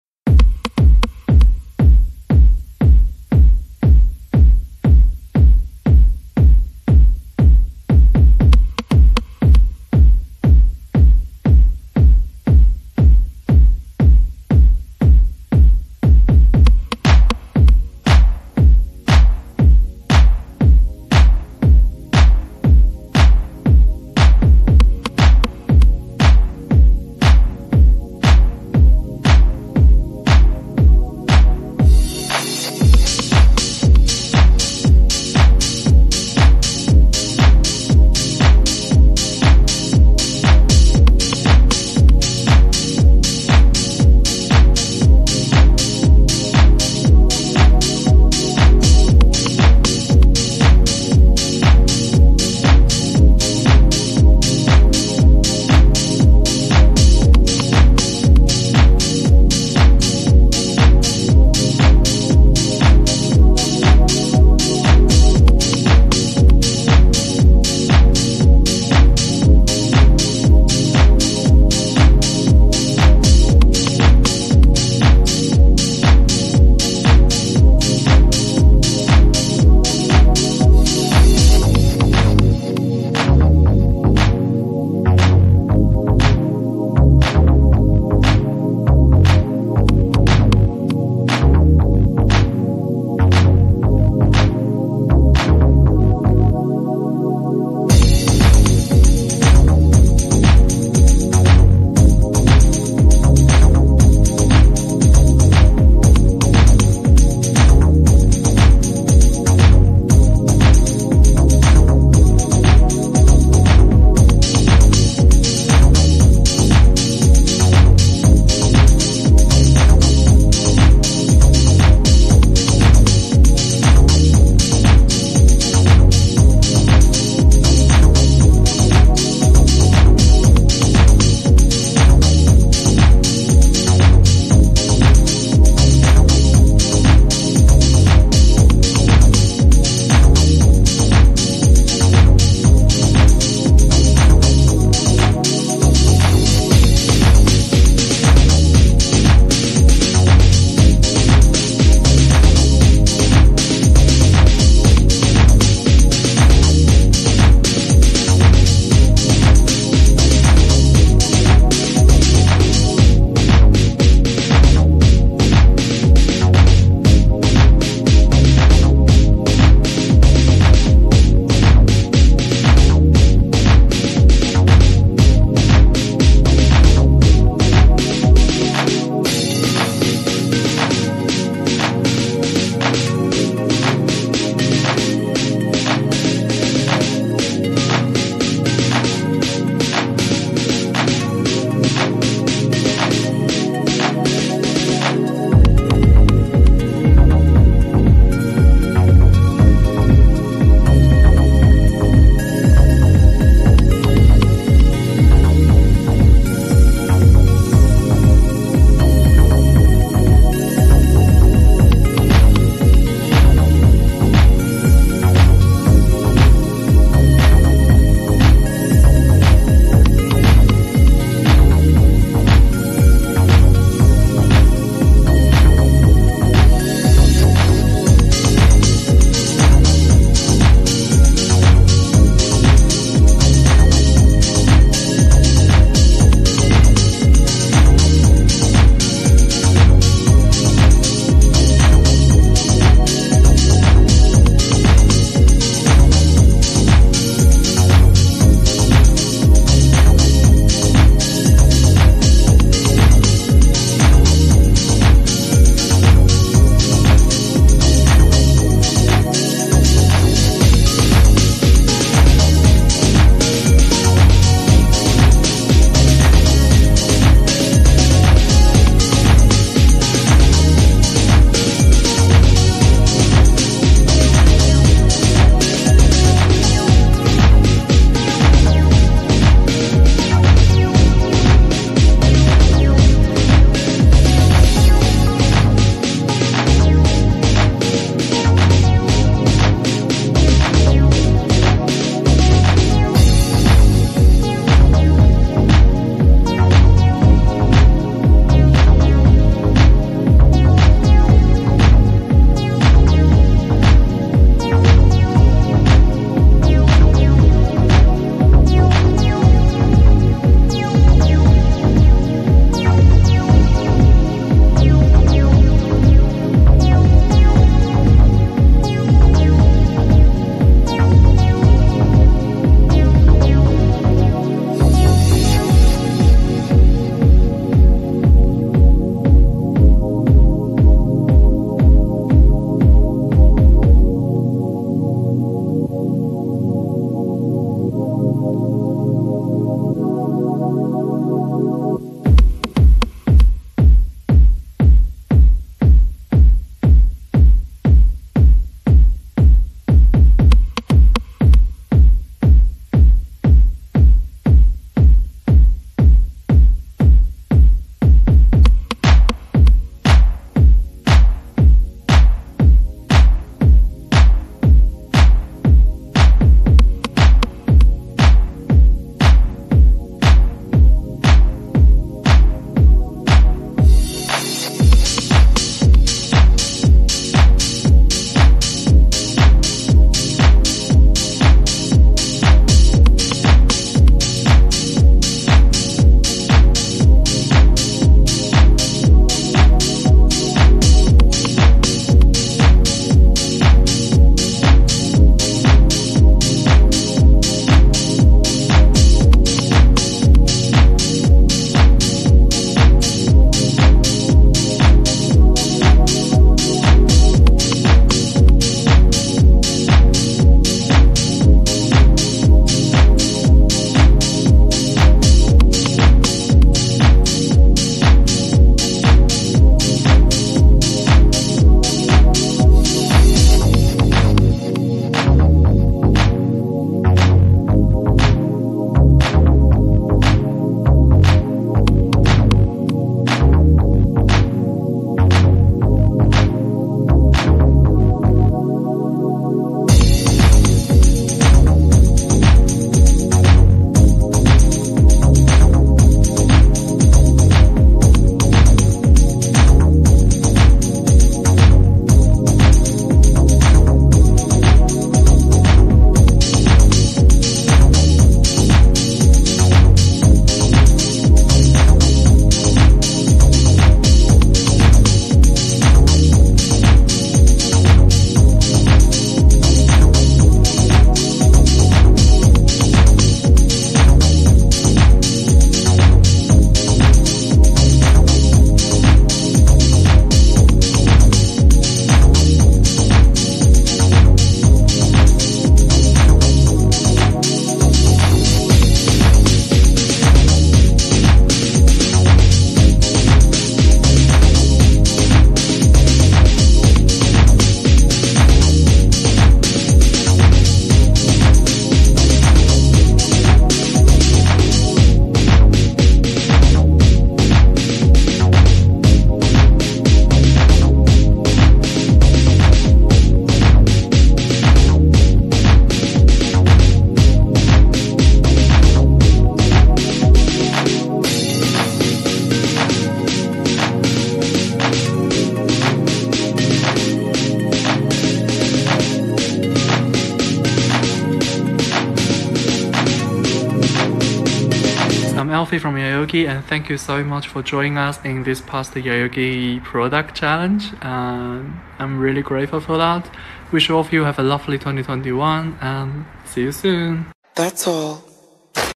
nailartist performance from around the world 🌍
today we have a girls around the world participating in the challange of @yayoge

check out their videos or instagram ⬇️

Name of Participants ~

2. @agriti_makeovers

3- @nail_art_amanda

8. @D.nailsbyme

10. @slayy_nailss (Garima)

11. @abbyd1v3na1ls

14. @nailartist_mette_eeg

15. @msdikshu
l

Check it out my Instagram  ⤵️